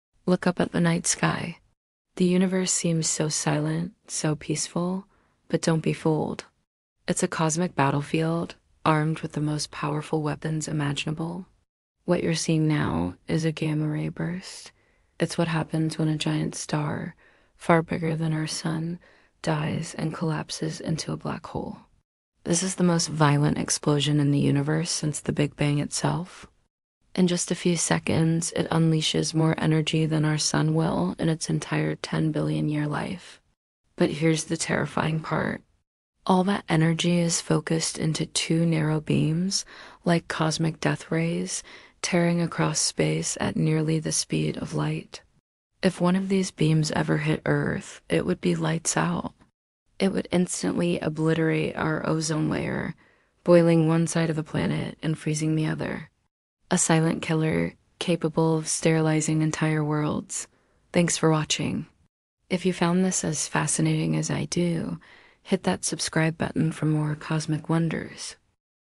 The Cosmic LASER That Could Sterilize Earth INSTANTLY 💥
Discover the Gamma-Ray Burst (GRB), the most powerful explosion in the universe. Born from collapsing stars, these cosmic lasers release more energy in seconds than our Sun will in its entire lifetime. What would happen if one hit Earth?

Part 1 of our new series, Cosmic Catastrophes.